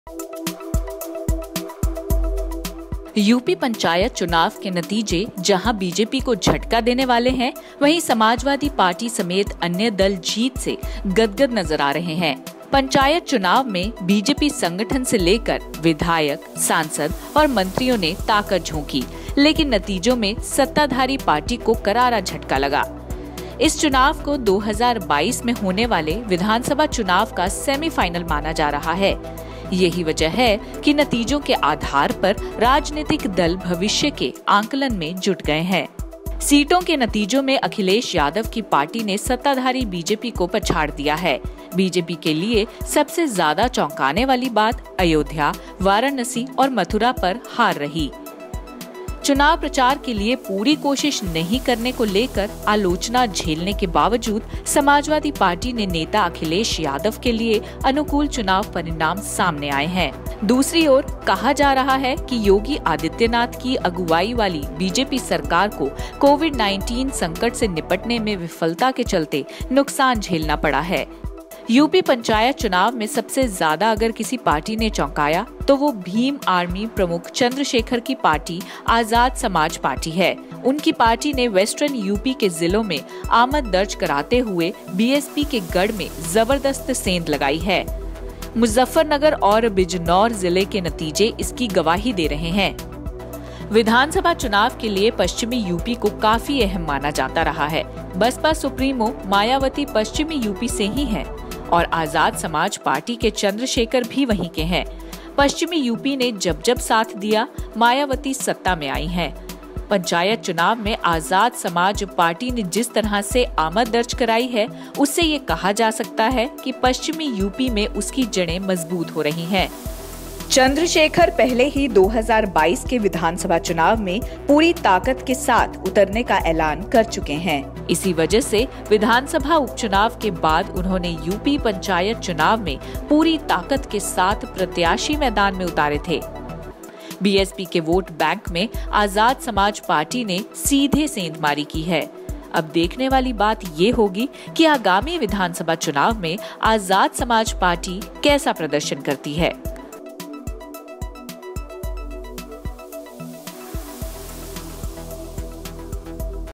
UP Panchayat Chunav में Chandrasekhar की पार्टी का शानदार प्रदर्शन, BJP-SP-BSP की बढ़ी धड़कन! |KADAK
उत्तर प्रदेश के पंचायत चुनाव में (Uttar Pradesh Panchayat Chunav 2021) सत्ताधारी बीजेपी पूरे जोर-शोर से उतरी थी. संगठन से लेकर विधायक (MLA), सांसद (MP) और मंत्रियों ने चुनाव में ताकत झोंकी. लेकिन नतीजों में बीजेपी कोे करारा झटका लगा है. अयोध्या (Ayodhya), काशी (Kashi) और मथुरा (Mathura) के नतीजे बीजेपी के लिए चौकाने वाला रहा. अयोध्या जिला पंचायत की 40 में से 24 सीटों पर मुख्य विपक्षी पार्टी सपा (Samajwadi Party) जीती है. वाराणसी (Varanasi) में भी 15 सीटों पर सपा समर्थित जीते हैं. इस चुनाव को 2022 में होने वाले विधानसभा चुनाव (UP 2022 Assembly Polls) का सेमीफाइनल माना जा रहा है. यही वजह है कि नतीजों के आधार पर राजनीतिक दल भविष्य के आकलन में जुट गए हैं. यूपी पंचायत चुनाव में सबसे अधिक भीम आर्मी प्रमुख चंद्रशेखर (Bhim Army Chief Chandrashekhar) की पार्टी आजाद समाज पार्टी (Azad Samaj Party) ने चौंकाया है. उनकी पार्टी ने वेस्ट यूपी (West UP) के जिलों में आमद दर्ज कराते हुए बसपा (BSP) के गढ़ में जबरदस्त सेंध लगाई है. मुजफ्फरनगर और बिजनौर जिले के नतीजे इसकी गवाही दे रहे हैं.

The Samajwadi Party is emerging as the frontrunner in the Uttar Pradesh rural polls with counting trends indicating on Wednesday that at least 742 candidates for zilla panchayat membership backed by the opposition party are either leading or have won the bellwether elections, compared to around 679 for the ruling Bharatiya Janata Party. Analysts see the polls, held amid a Covid-19 surge in the state, as a key test of the Yogi Adityanath-led BJP government’s popularity and an opportunity for opposition parties to gauge the public mood, particularly as the exercise comes against the backdrop of persisting farmers’ protests in the country that have also gripped parts of UP.​